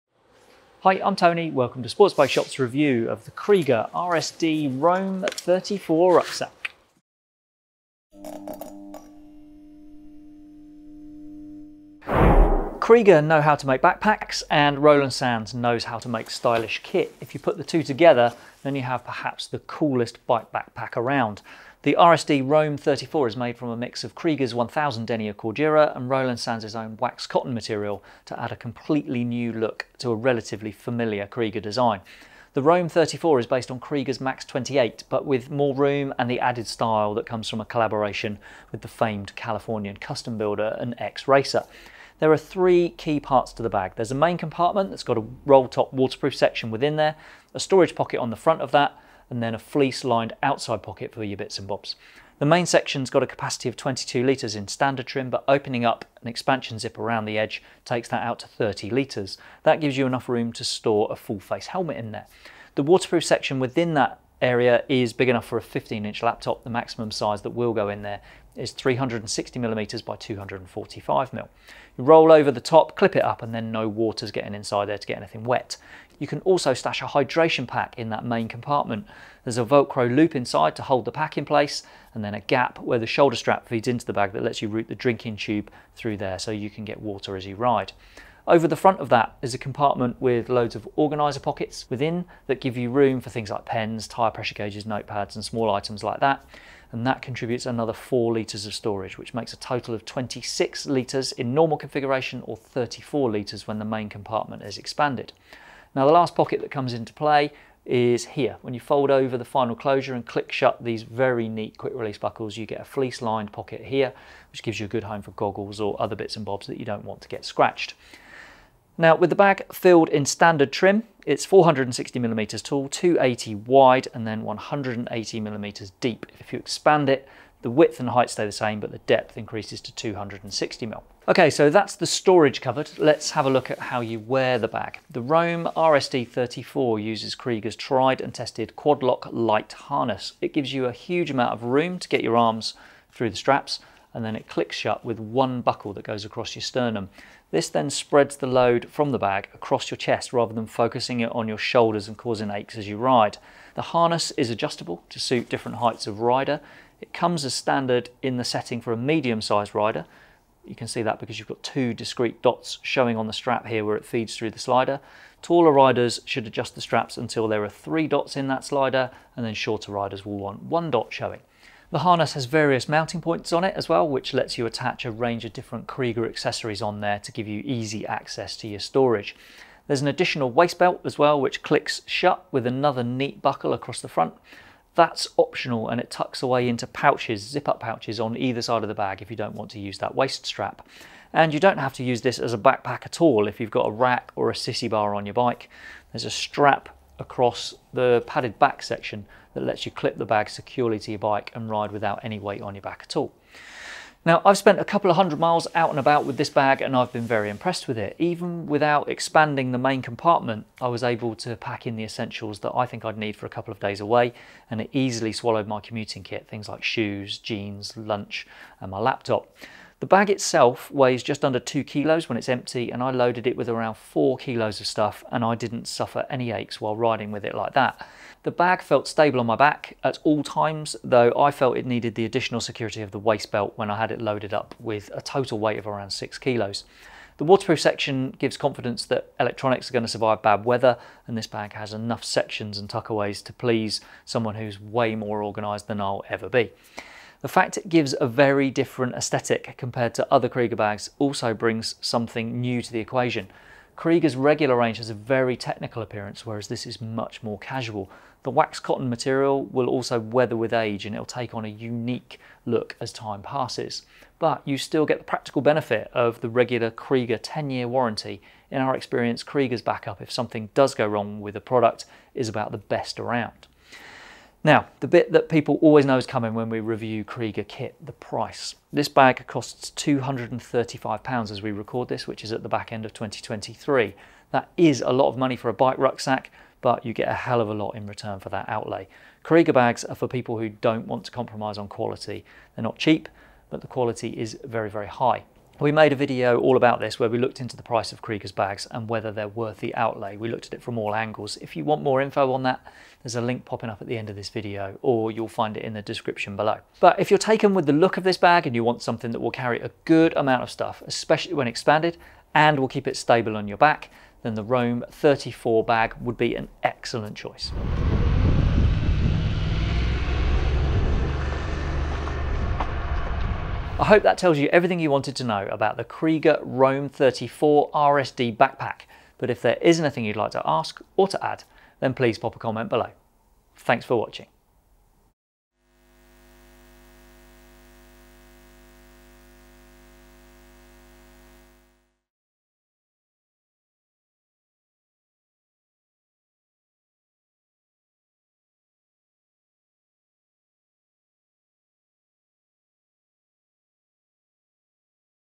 Kriega Roland Sands Design Roam 34 backpack review
Mix Kriega’s luggage knowhow with Roland Sands’ artistic flair and you get a seriously cool riding rucksack.

The Kriega RSD Roam 34 starts out as a roomy 26-litre rucksack that’ll hold enough stuff for an overnight stop. Whip the expansion zip open and it’ll grow into a 34-litre bag big enough to swallow a full-face helmet.

With a waterproof inner section for a laptop, room for a hydration pack and Kriega’s Quadloc Lite harness to help prevent shoulder and backache, both style and practicality are covered.

Find out all the details and get our take on it by watching our road test review.